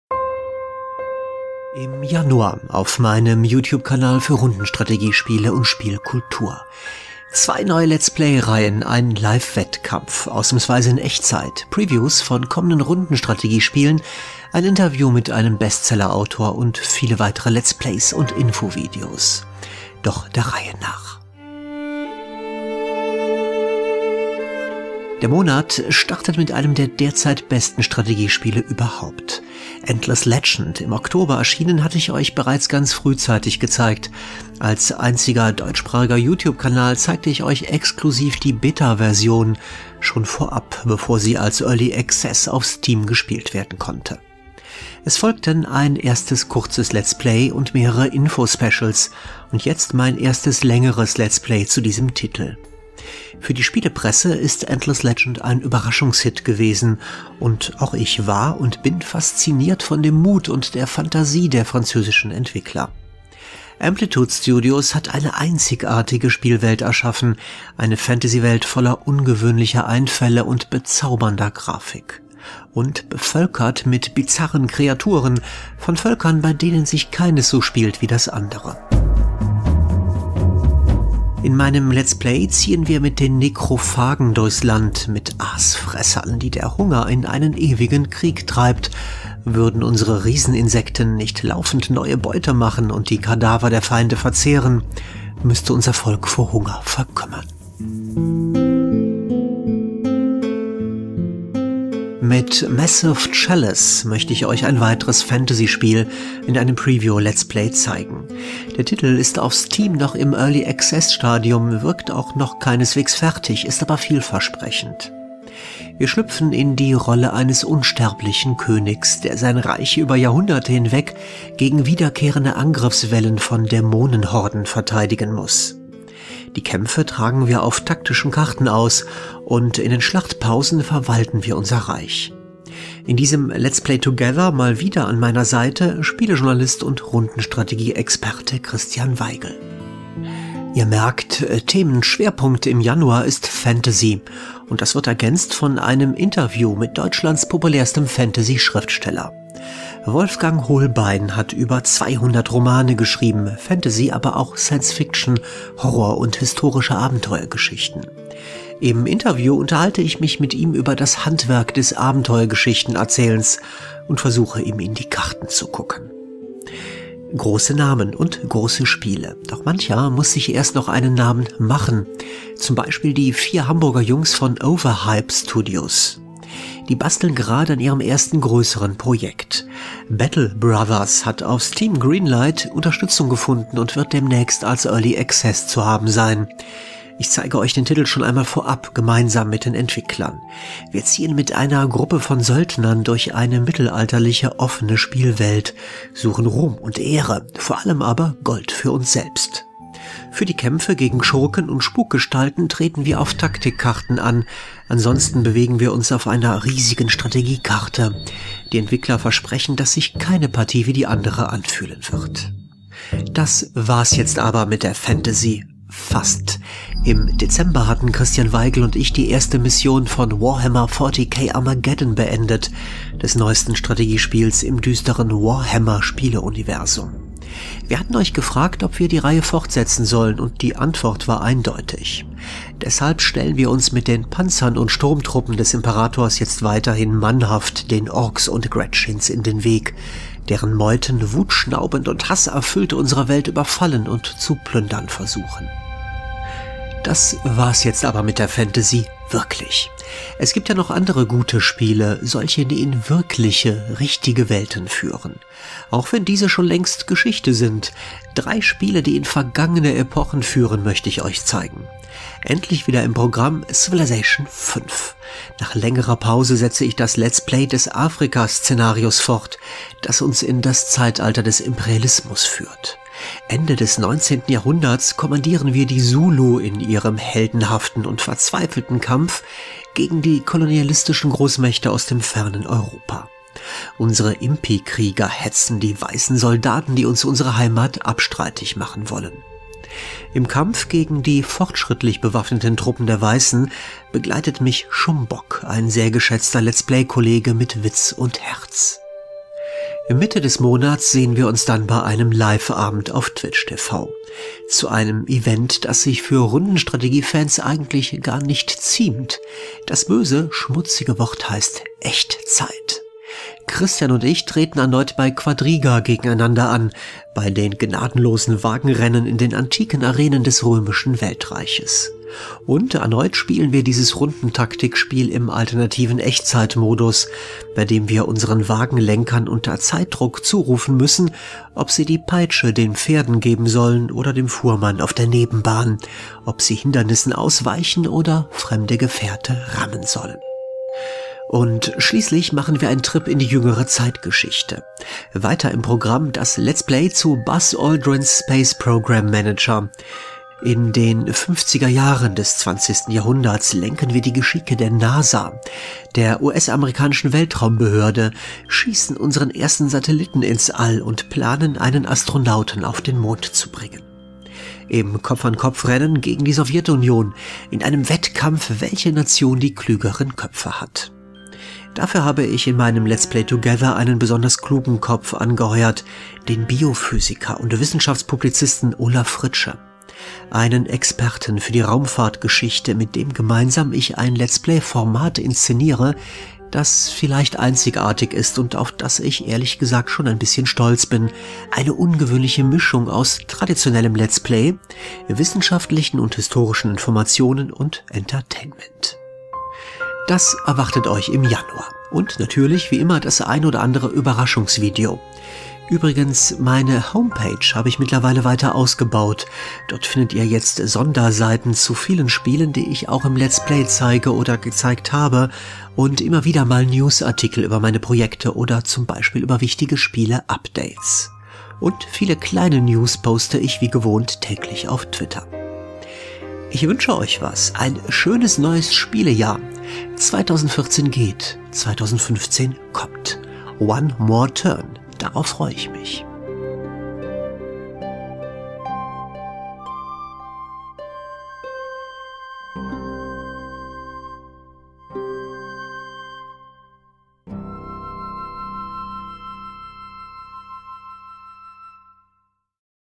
Programm im Januar 2015
Im Januar auf meinem YouTube-Kanal für Rundenstrategie und Spielkultur: Zwei neue Fantasy-Let's-Plays zu dem bezaubernden "Endless Legend" sowie dem Early-Access-Titel "Massive Chalice". Ein Preview mit den Hamburger Indie-Entwicklern von "Battle Brothers". Außerdem: Ein Interview mit Bestsellerautor Wolfgang Hohlbein. Antike Wagenrennen beim Liveabend mit "Qvadriga" und eine neue Mission beim Let's Play Together zu "Warhammer 40k: Armageddon", jeweils gemeinsam mit Spielejournalist Christian Weigel. Wieder im Programm: das Afrika-Szenario von "Civilization V", zusammen mit Shumbok. Und weiterhin das ungewöhnliche Let's Play Together zu "Buzz Aldrin's Space Program Manager", Seite an Seite mit Biophysiker Olaf Fritsche. Und einiges mehr!